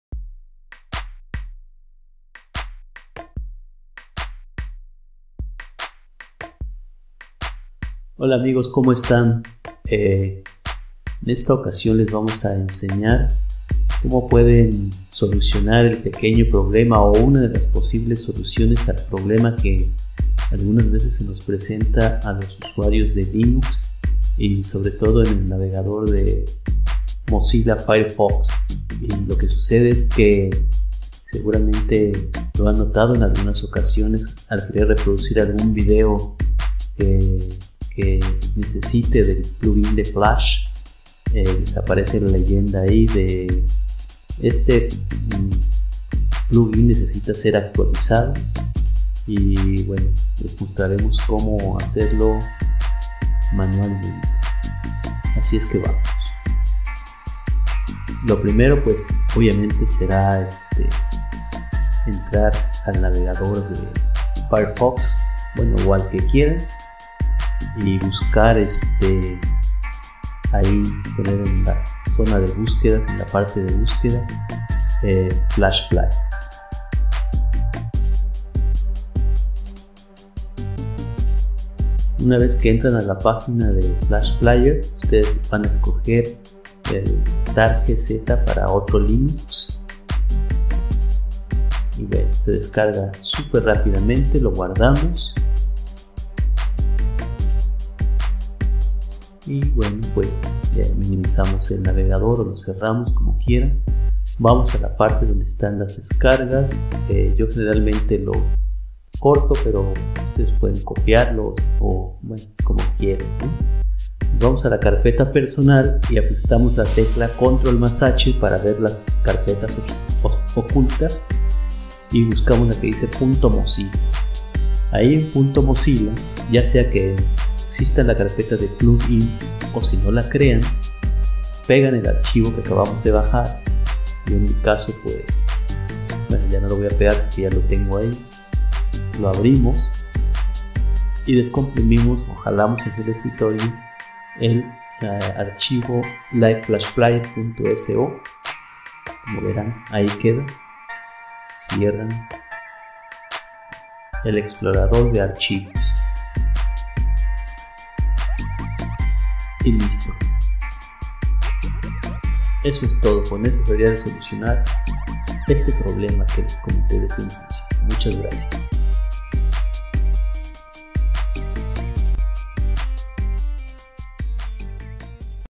Problema con plugin de Flash en linux
Una de las soluciones al problema que se presenta con el plugin de flash en Linux. Algunas veces al querer reproducir un video en firefox desde Linux nos aparece la leyenda "el plugin de flash necesita ser actualizado". Aquí te presentamos una solución.

2. Corta o copia el archivo descargado.
3.- Ve al explorador de archivos, carpeta personal.
4.- Utiliza ctrl + h para ver  las carpetas ocultas.
5.- Busca .mozilla y abréla
6.- busca o crea la carpeta plugins.
7.- Pega el archivo tar. gz.
8.- Abre el archivo tar.gz y extrae el archivo libflashplayer.so

Ojala te sirva esta información

Somos una asociación civil que difundimos el uso de Linux y del software libre, así como arte, cultura, activación física y la preservación del medio ambiente.